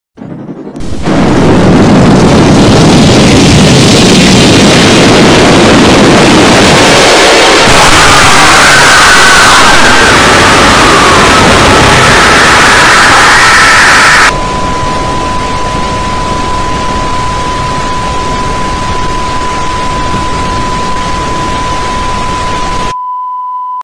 (HEADPHONES WARNING) All WGBH Boston G Major Screamers at Once
Before watching this, please lower your volume before watching this.

A total of 23 WGBH G-Major jumpscares